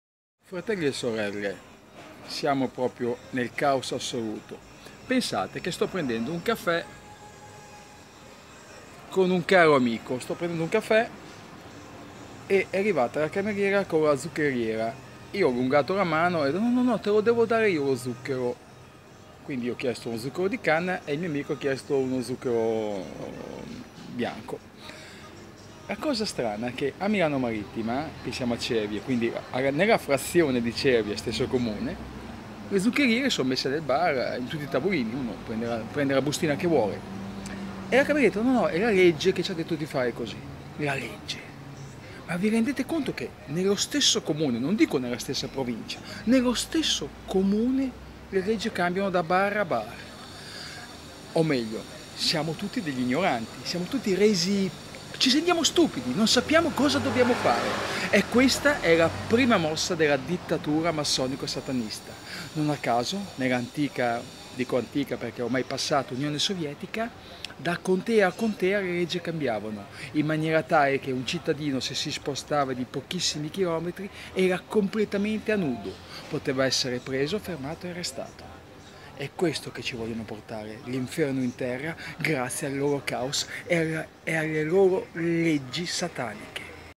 Al Bar infrangevo la legge senza saperlo: Loro vogliono dominarci con "Caos"
La Conoscenza rende liberi, ma Loro fanno di tutto per creare il Caos;
Vi presento la mia esperienza al bar con una bustina di zucchero, mente con un amico infrangevo la legge senza saperlo
( Padre David )